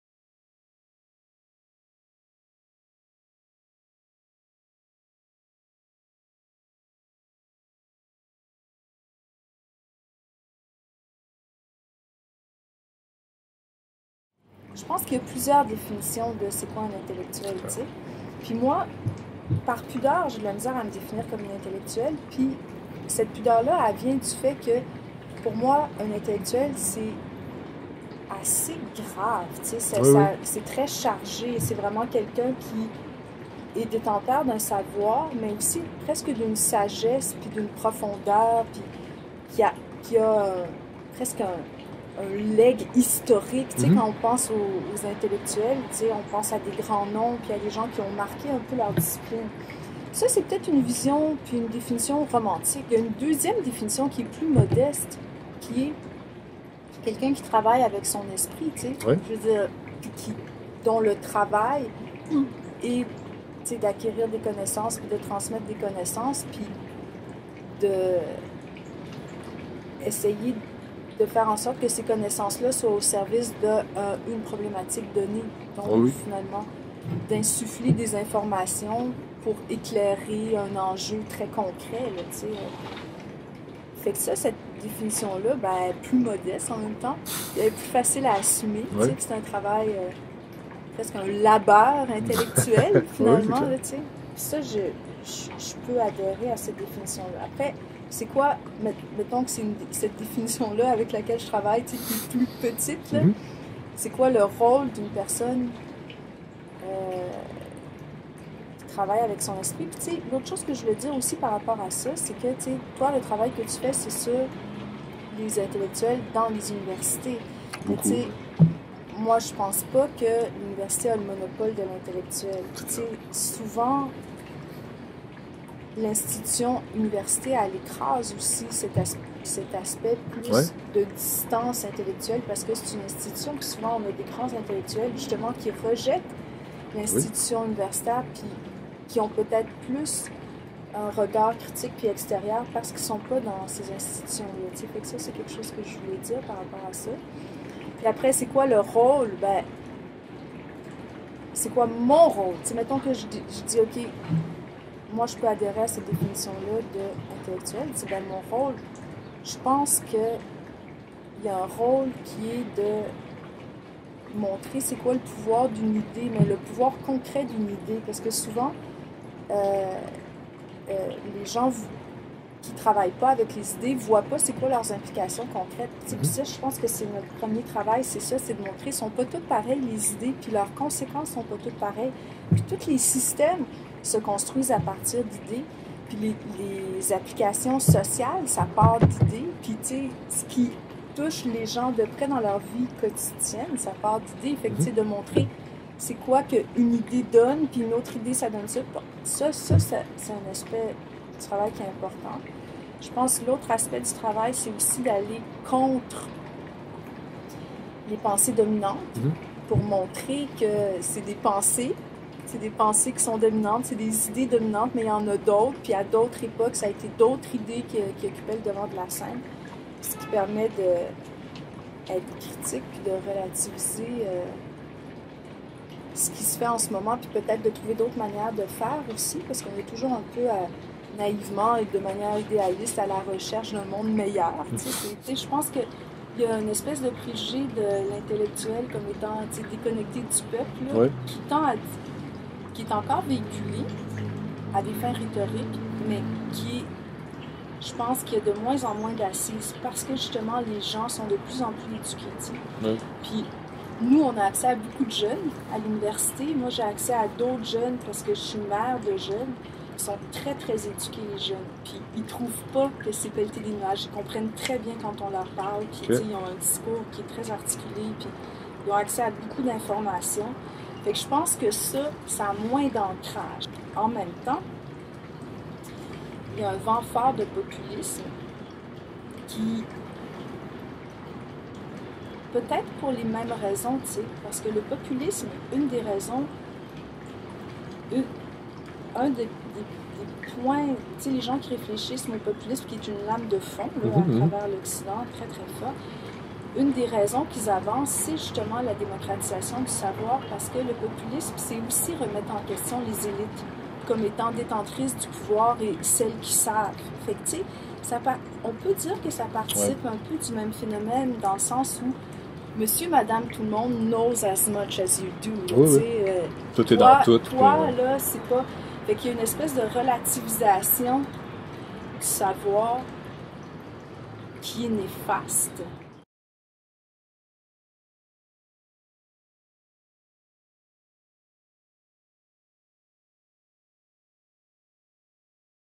Julie Desrosiers - Figure de l'intellectuel
Cette vidéo a été réalisée dans le cadre du projet "Paroles de chercheur-es" qui a permis de recueillir les impressions de 41 chercheur-es québécois-es sur quatre thématiques liées à l'importance et la fonction de la recherche indépendante dans la société, soient : L'université, La recherche indépendante, La figure de l'intellectuel et Menaces et périls.